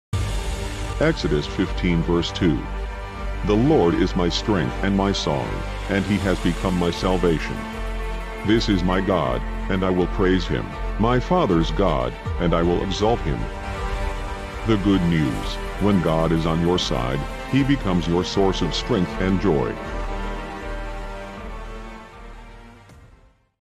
BEST FOR YOU & ME: EXODUS 15:2
EXODUS 15:2.

"The Lord is my strength and my song, and he has become my salvation; this is my God, and I will praise him, my father's God, and I will exalt him"

The Good News: When God is on your side, He becomes your source of strength and joy.